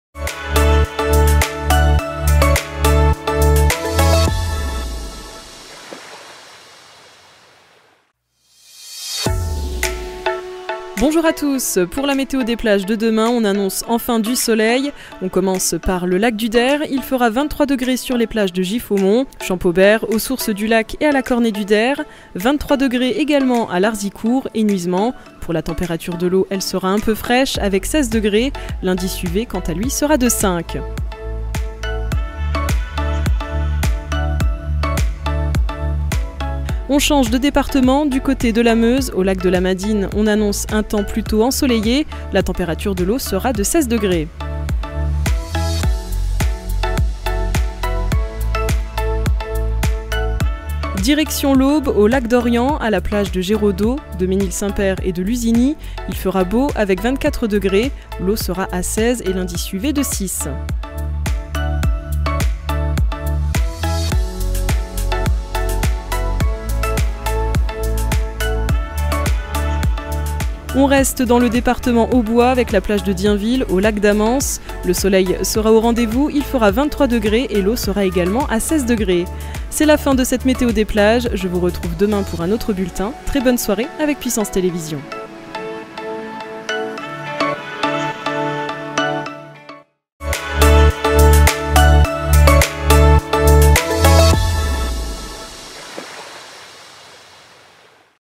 La météo des plages | Mercredi 07 juillet 2021
Vos prévisions météo sur les lacs de la région du 07 juillet 2021.